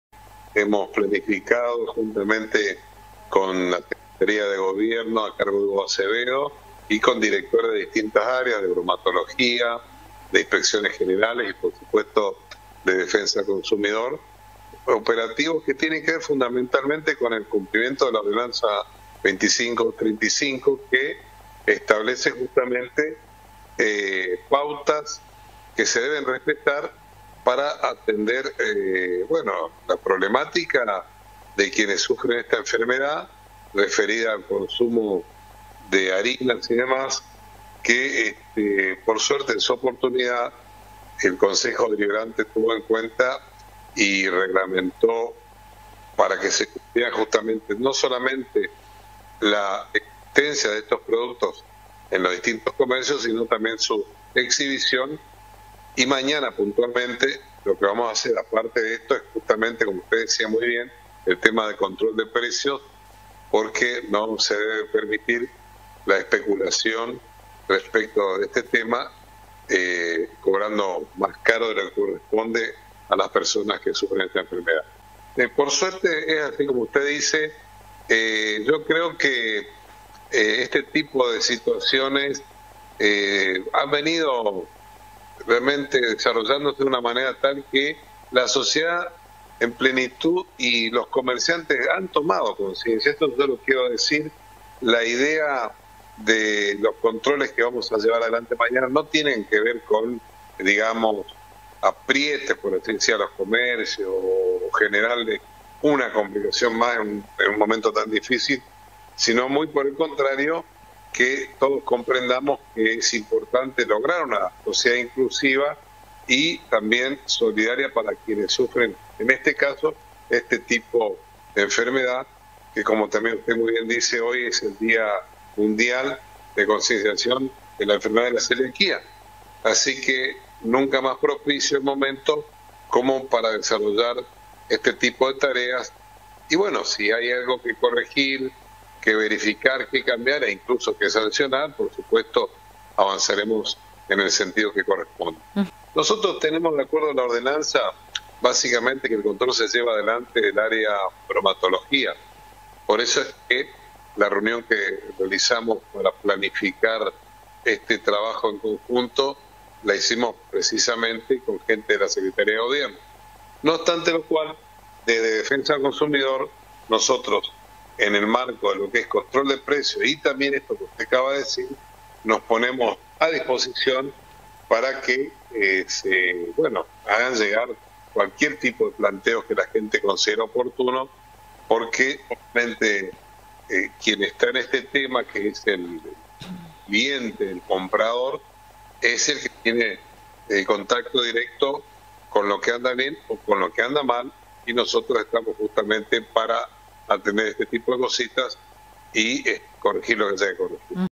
CONTROL DE COMERCIALIZACIÓN DE PRODUCTOS PARA CELÍACOS
La Municipalidad de Resistencia dispuso controles específicos para verificar el cumplimiento de la ordenanza 12.325 que establece regímenes especiales para la comercialización de productos para celíacos. Hoy 5 de mayo es el día de esa enfermedad. El subsecretario de Industria y Comercio, Sergio Vallejos,  junto a la  Directora General de Bromatología y los Directores Martin Paz y Georgina Smith, planificaron los controles que junto a Defensa al Consumidor, con Natalia Pontel y Jonathan Franco, comenzarán mañana mismo en diversos comercios de esta ciudad
Sergio Vallejos indica que la Municipalidad también recibirá consultas, reclamos y sugerencias de los vecinos y consumidores de estos productos para poder optimizar dichos operativos.
SERGIO VALLEJOS - SUBSECRETARIO DE INDUSTRIA Y COMERCIO